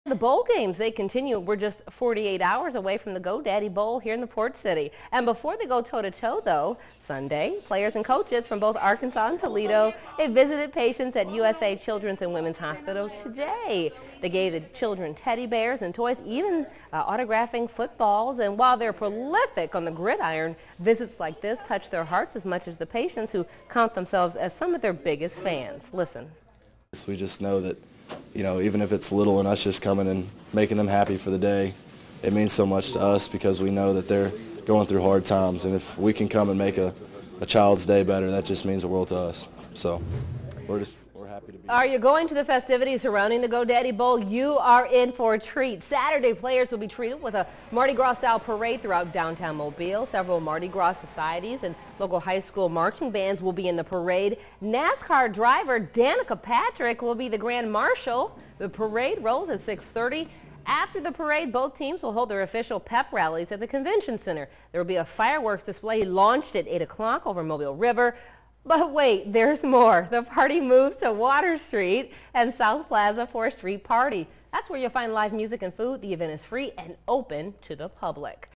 Team Visits Hospital Friday; Go Daddy Parade Sat.
MOBILE, Ala. (WPMI) The Go Daddy Bowl is Sunday in the Port City. But before the big game some of the players visited some of the youngest football fans in the area. Players and coaches from both Arkansas and Toledo visited patients at USA Children's and Women's Hospital Friday.

They gave the children teddy bears, toys and even some autographed footballs. While they are profilic on the gridiron, visits like the one Friday touch their hearts as much as the patients who count themselves as some of their biggest fans.

"We know it's something little and us coming and making them happy for the day. It means so much to us because we know that they're going through hard times. If we can come and make a child's day better, that just means the world to us," said outside linebaker Benjamin Gallagher.

There's more bowl action set to take place this weekend. Players will be treated with a Mardi Gras style parade Saturday through downtown Mobile. Several Mardi Gras societies and local high school marching bands will be in the parade.

Nascar driver Danica Patrick will serve as the Grand Marshal. The parade rolls at 6:30 p.m.

Right after the parade, both teams will hold their official pep rallies at the Convention Center. You can catch a fireworks display at 8 p.m. over Mobile River. Then the party moves to Water Street and South Plaza for a street party. There will be live music and food. The event is free to the public.